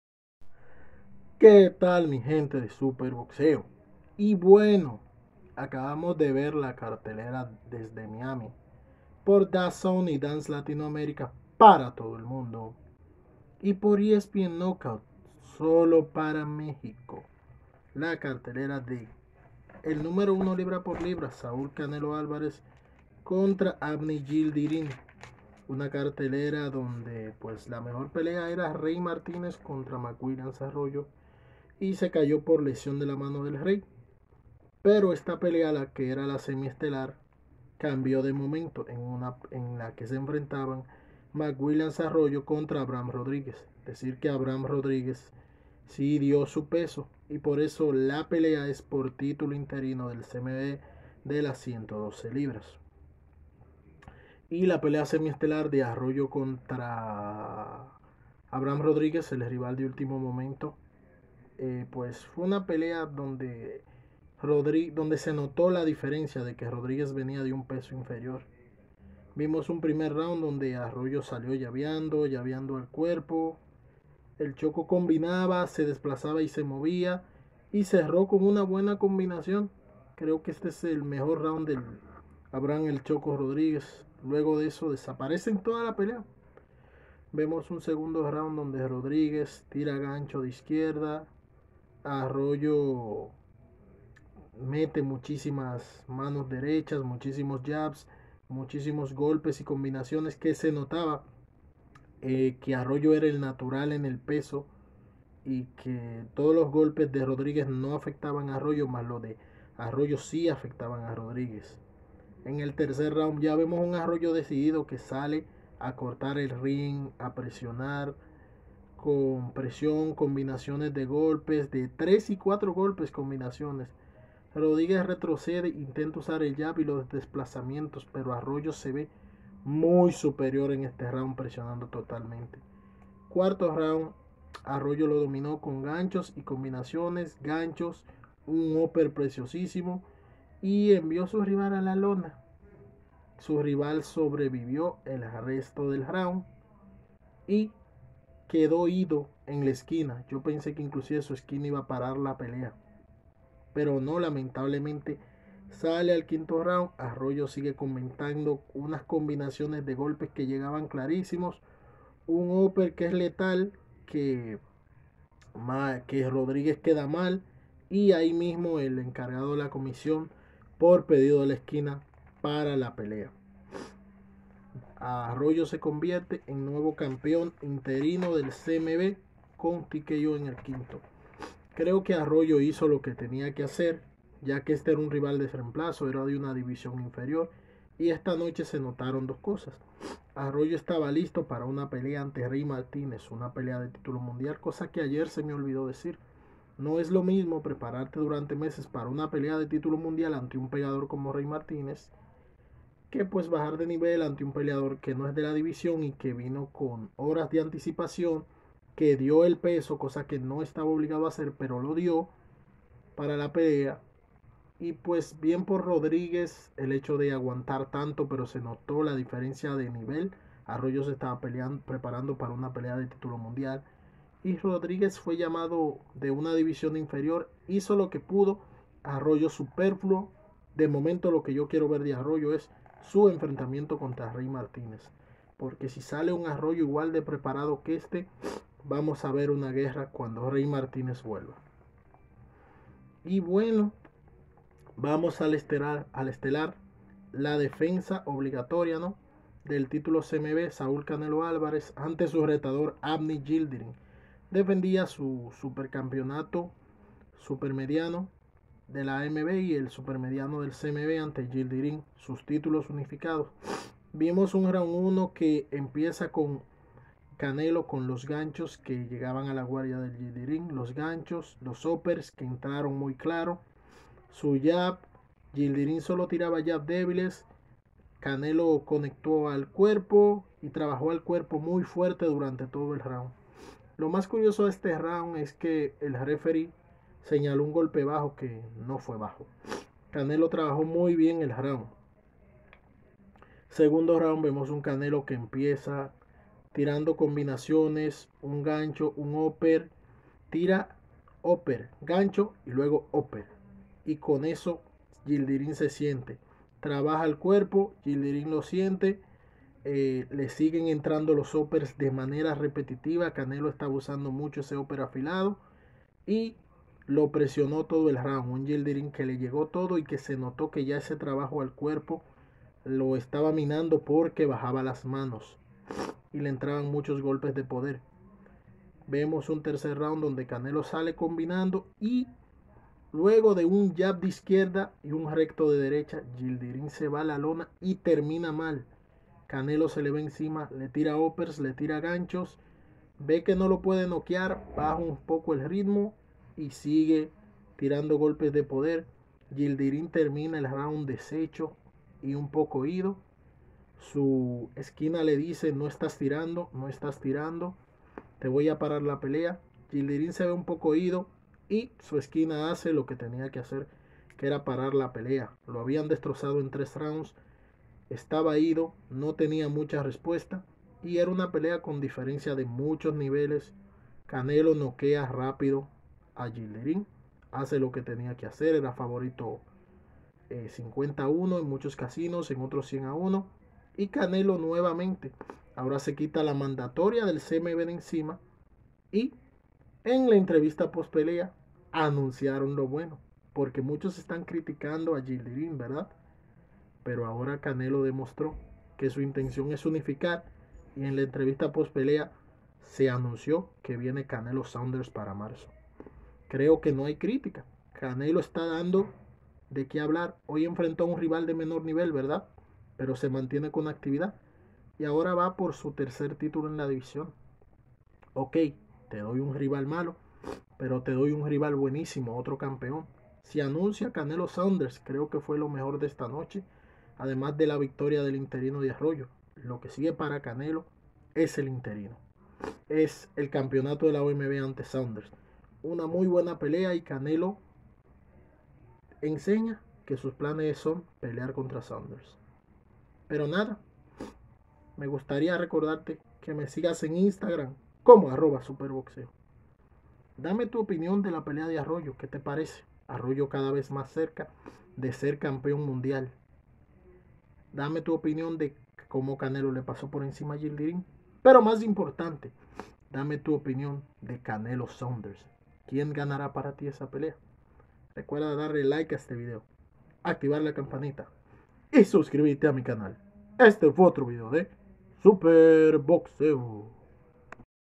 Canelo Alvarez vence por TKO EN 3 Avni Yildirim Y Anuncia pelea con Billy Joe Saunders
TE GUSTARÁN***

Entrevista Con Jackson Mariñez habla de Rolando Romero Y su Proximo Rival Richard Commey